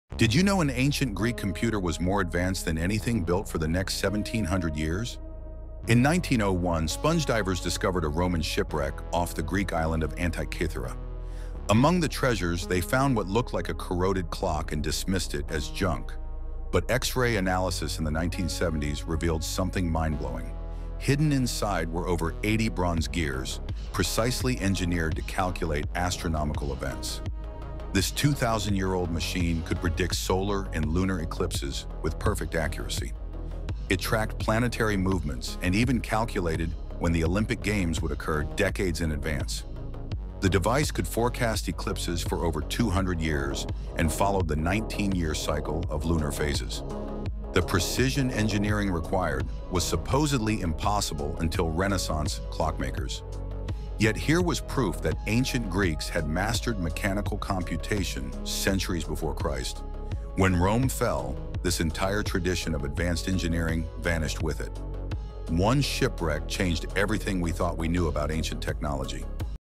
Ancient Greeks Built a Computer 2000 Years Before We Thought Possible
Ancient computer more advanced than anything for 1700 years 🤯 history ancient techn...